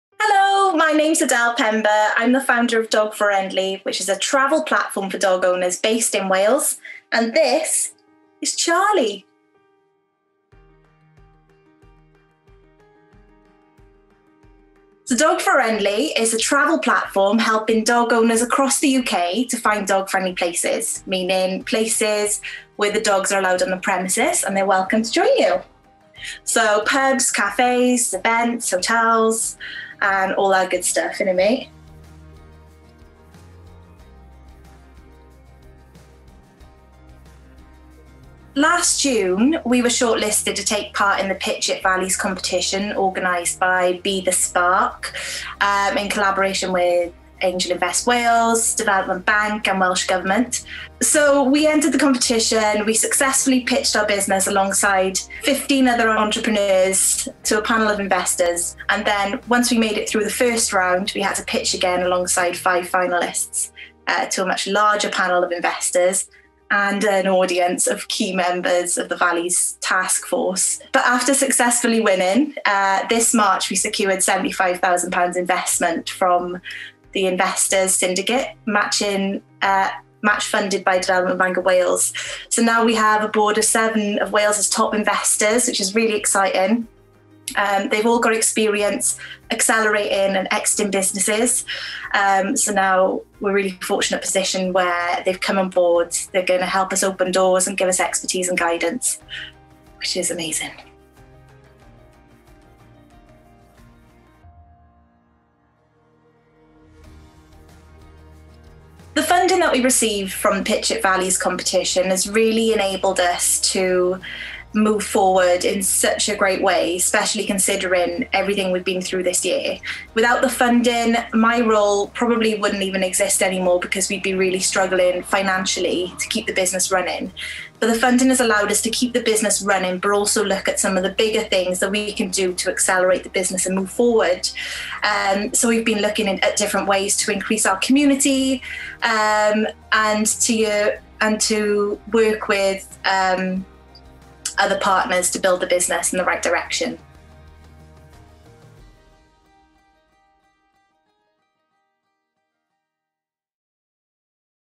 Our Valleys: Adele, Dog Furiendly / Ein Cymoedd: Adele, Dog Furiendly
Since 2017, the Valleys Taskforce have developed many schemes and initiatives to support communities in the South Wales Valleys. We spoke to Founder of Dog Furiendly Adele Pember about how winning the Pitch It Valleys event helped her business to grow.

Ers 2017, mae Tasglu’r Cymoedd wedi datblygu nifer o gynlluniau a mentrau i gynorthwyo cymunedau yng Nghymoedd y De. Fe wnaethom ni siarad ag Adele Pember, sefydlwr Dog Furiendly, ynglŷn â sut y gwnaeth ennill digwyddiad Pitch It Cymoedd ei helpu i dyfu ei busnes.